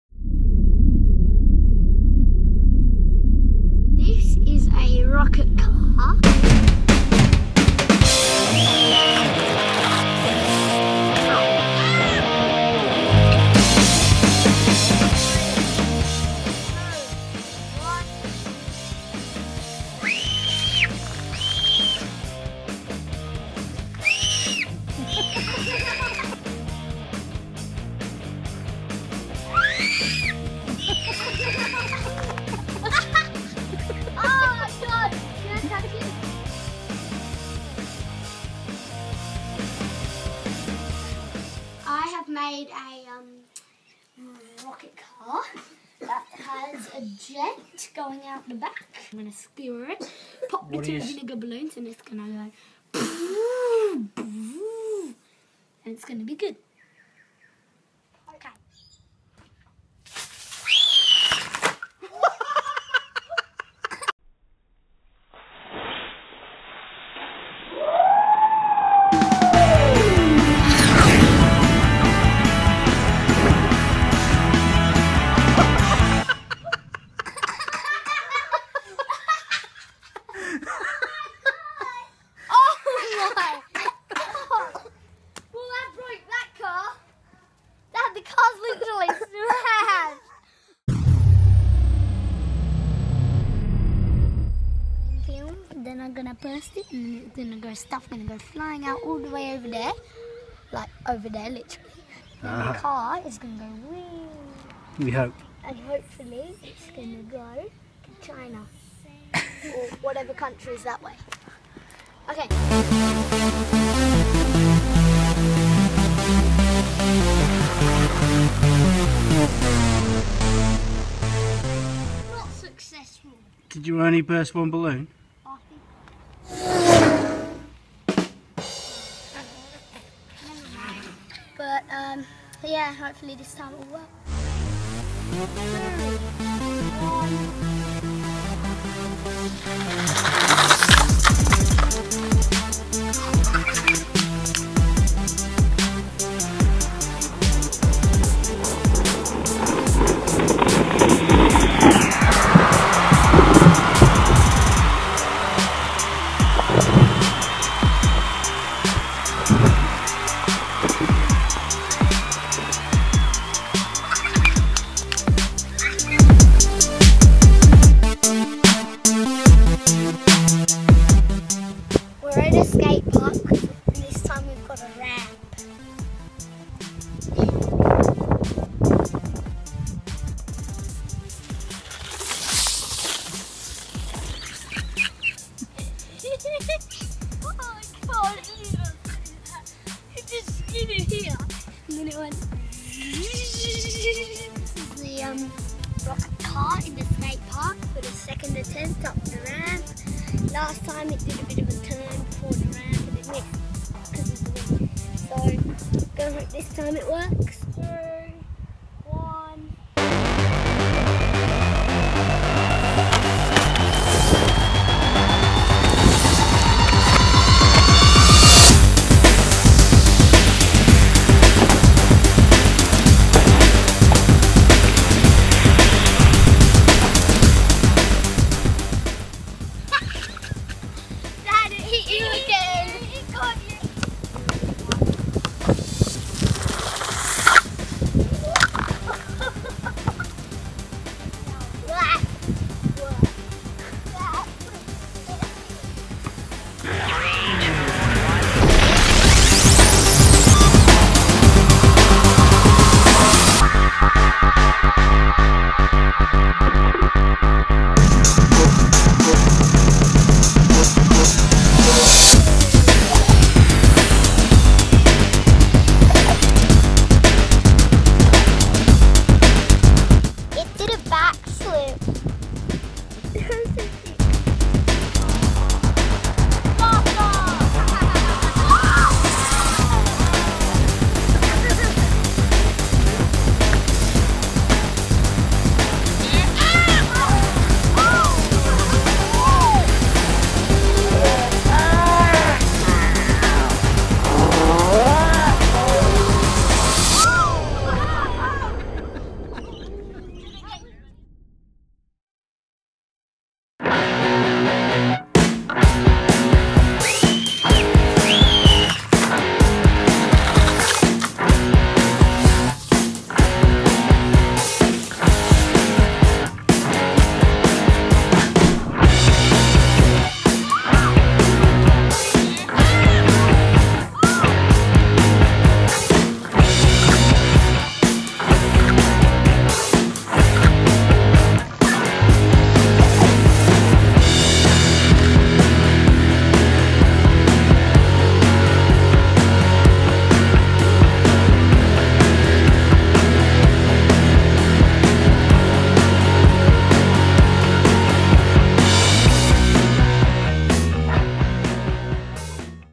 ✪ Rocket Car Fun! ✪ Home Made Rocket Car Takes Off Very Fast at the Skate Park. Fun Home Science
Great fun with a simple, cheap, home made rocket car.
Made with:
- Plastic bottle chassis
- Plastic Bottle Rocket Pack
- Vinegar
- Bicarbonate of Soda
- Water balloons (to keep liquids separate)
- Bottle top wheels
- Skewers for axels (through straws)
- And a bit of duct tape
- Skewer to burst water balloons

Total cost less than $1!
Total Fun? Priceless!

Well done William!